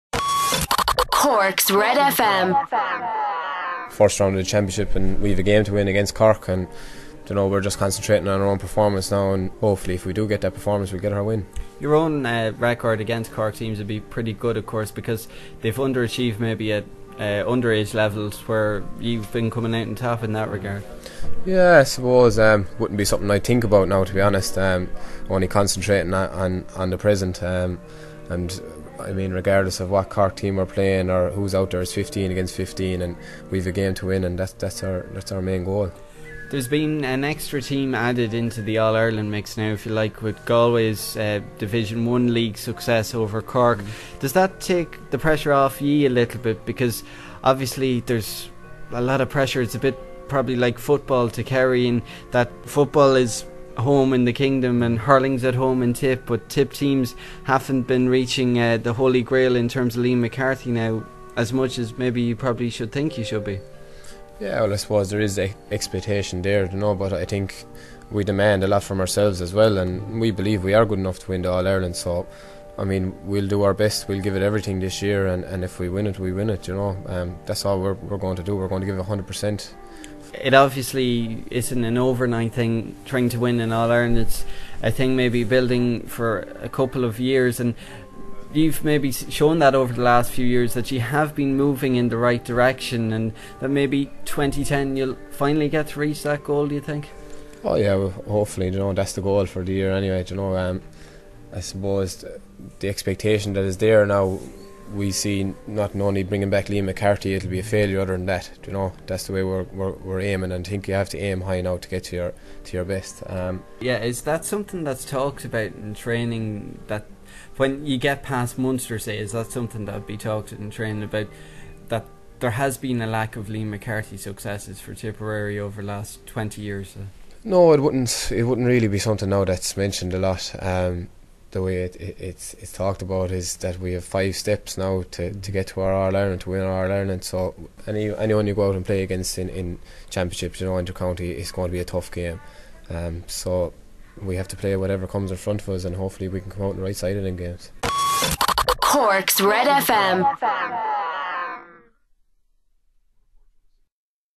Brendan Maher 290510
Tipperary's Brendan Maher previewing their meeting with Cork in the first round of the Munster SHC.